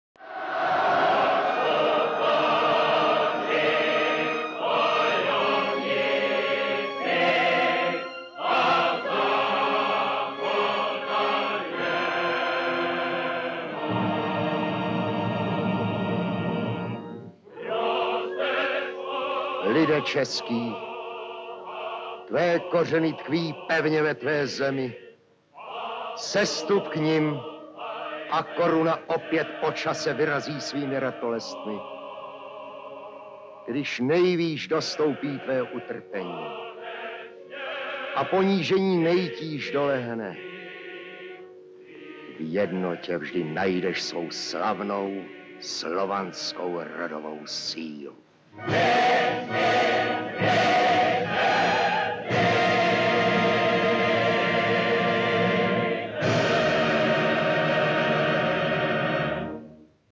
Jan Roháč z Dubé - soudobá dokumentace
V bitvě u Lipan byla zničena hlavní síla husitských vojsk, ti, kteří nepadli, byli buď upáleni nebo uvrženi do vězení. Mezi posledními byl i Jan Roháč z Dubé. Páni, díky jejichž komplotům byl na trůn dosazen Zikmund, přemlouvali i Roháče, aby se smířil. Ten však tuto nabídku nepřijal a odešel s posledními Tábority na hrad Sion a Zikmundovi poslal ke korunovaci list, ve kterém ho prohlásil za „škůdce země“. Zikmund proti němu posílá vojsko. To po dlouhém obléhání hrad Sion obsadilo, zapálilo a husity zajalo. Zikmund nad nimi vyřkl soud a nechal všech 54 zajatců pověsit. V národě však znovu ožily husitské ideály a to vedlo ke vzpouře lidu proti Zikmundovi, který byl donucen ze země uprchnout.